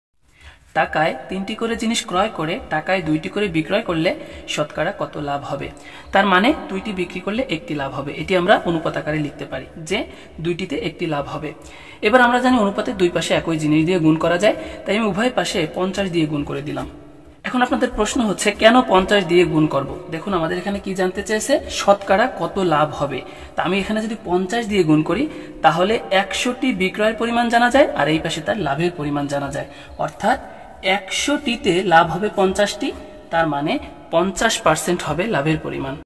সহজে লাভ ক্ষতি অংকের সমাধান।
1.লাভ ক্ষতির অংক সহজ সমাধান।
2. অনুপাত সমানুপাত।